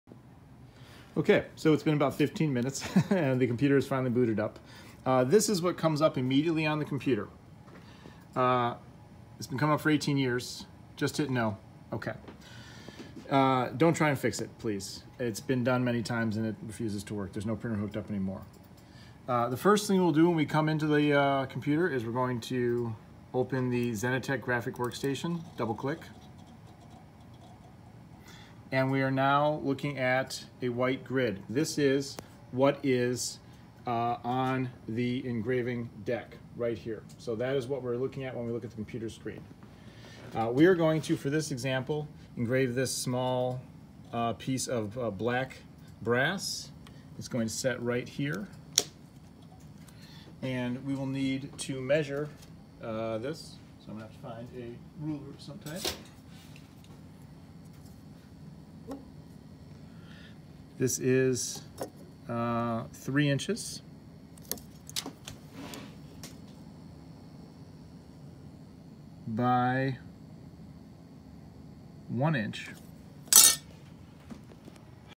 Xenetech version 3.11 controlling an XOT 16x25 pt 1
running a very early version of xenetech control software on windows 98